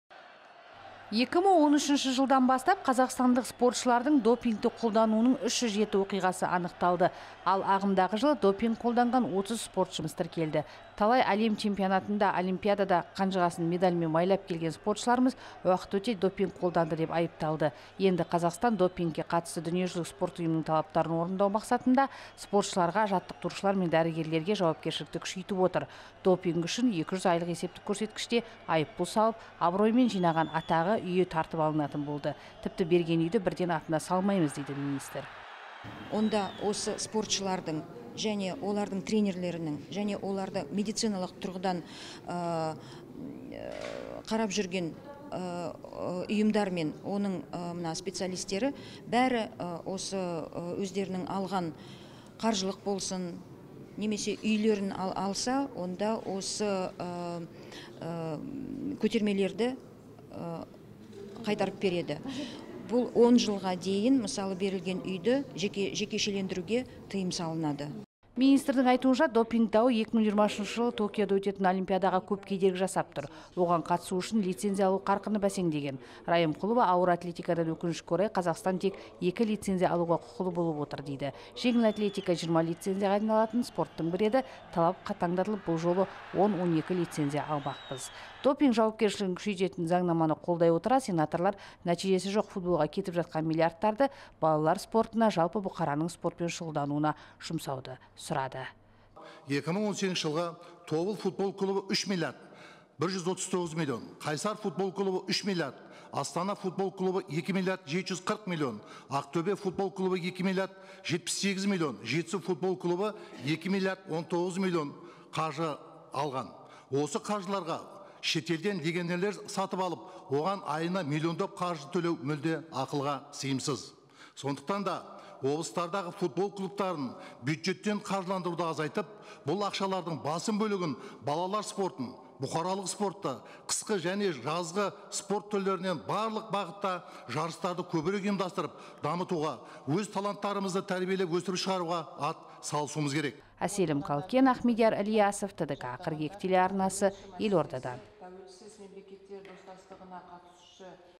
Спортшыларға талап күшейтілді
Сенат  допингке қарсы ережелерді бұзғаны үшін жауапкершілікті қатаңдатуға, Ұлттық допингке қарсы ұйымның мәртебесін күшейтуге бағытталған денешынықтыру және спорт мәселелері бойынша айтарлықтай өзгерістер енгізілген заң жобасын қабылдады. Енді допингмен ұсталған спортшылар мен олардың жаттықтырушылары, дәрігерлері жауапқа тартылады.  Спортшылар жарыстан шеттетіліп, құрама және штаттық командалар құрамынан шығарылады. Бұдан бөлек баспанасы мен сыйақысынан айырылады. Ал допинг үшін 200 АЕК  айыппұл салынады.